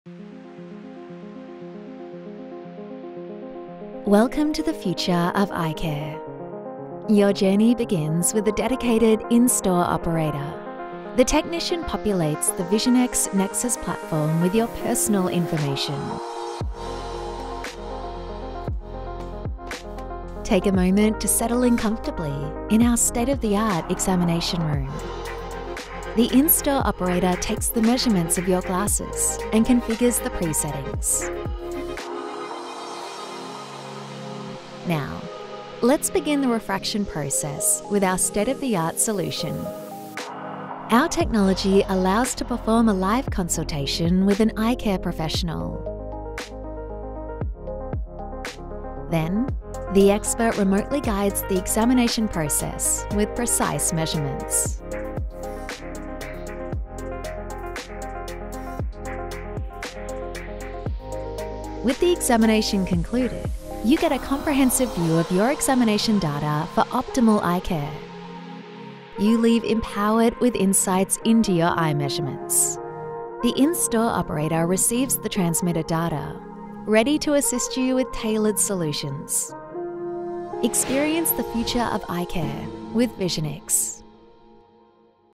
Refração Remota com Eye Refract da Visionix: Precisão e Conforto
Descubra como o Eye Refract da Visionix está revolucionando a refração remota. Com tecnologia avançada, obtenha medições de refração precisas e confiáveis, garantindo ao mesmo tempo um conforto ótimo para seus pacientes. Explore os benefícios desta solução inovadora que redefine os padrões da optometria. Ideal para profissionais que buscam modernizar sua prática enquanto aprimoram a experiência do paciente.